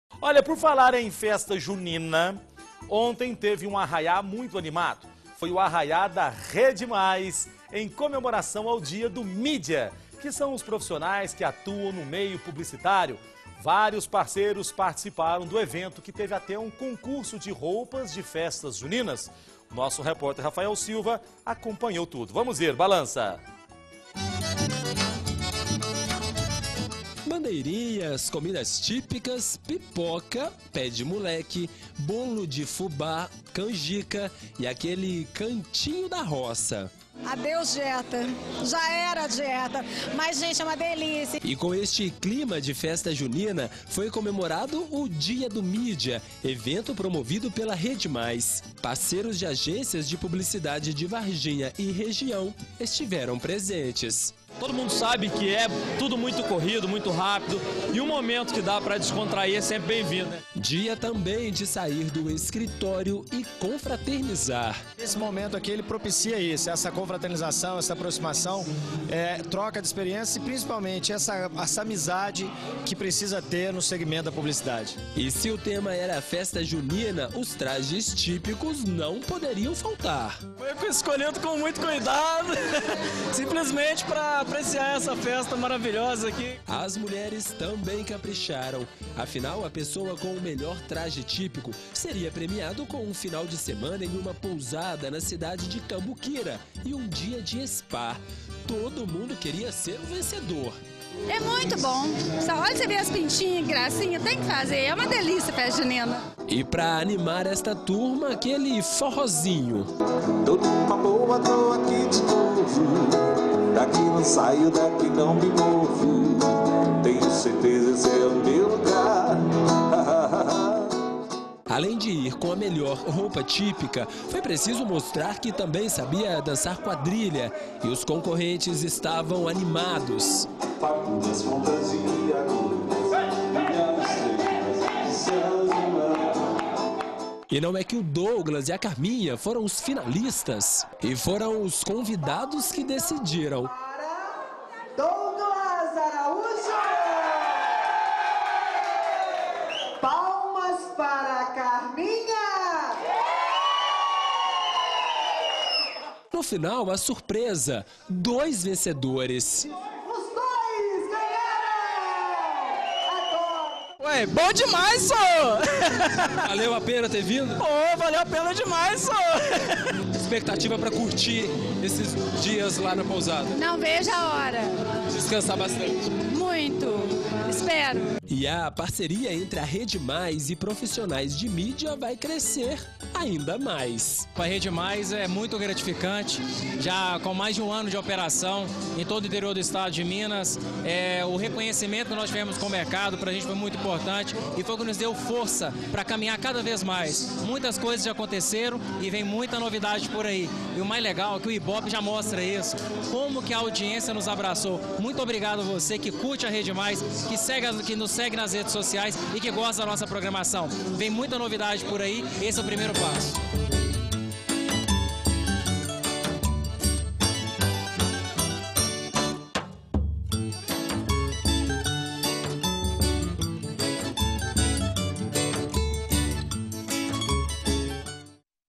Dia do mídia: “Arraiá” da Rede Mais comemora a data
A Rede Mais promoveu nesta segunda-feira (19) um “arraiá” bastante animado. O evento foi em comemoração ao Dia do Mídia, que são os profissionais que atuam no meio publicitário. Vários parceiros participaram do “arraiá”, que teve até um concurso de roupas típicas da festa junina.